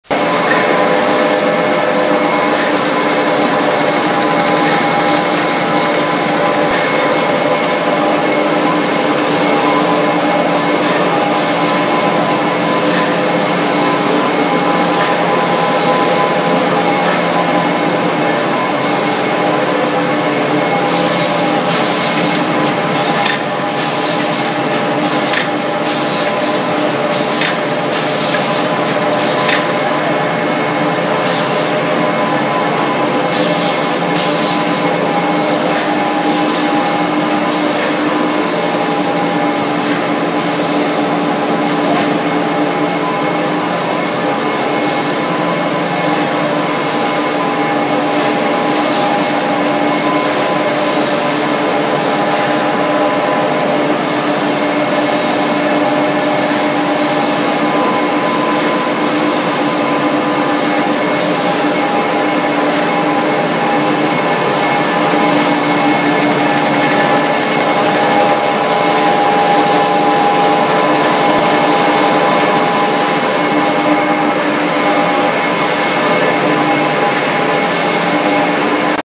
А4 резки бумаги，A4 PAPIER MACHINE DE COUPE，A4 DE CORTE DE PAPEL DE LA MÁQUINA，
or wechat (whatsapp): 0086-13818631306,prefer wechat

А4 резки бумаги
A4 Paper Cutting and packing machine
30-35 пачек / мин
ширина бумаги катушка:1060mm

A4 PAPIER MACHINE DE COUPE
Bobine de papier WIDTH: 1060mm
vitesse: 30-35 rames / min

A4 DE CORTE DE PAPEL DE LA MÁQUINA
PAPEL REEL ANCHO: 1060mm
velocidad: 30-35 resmas / min

Alternative E.C.H WILL and Bielomatic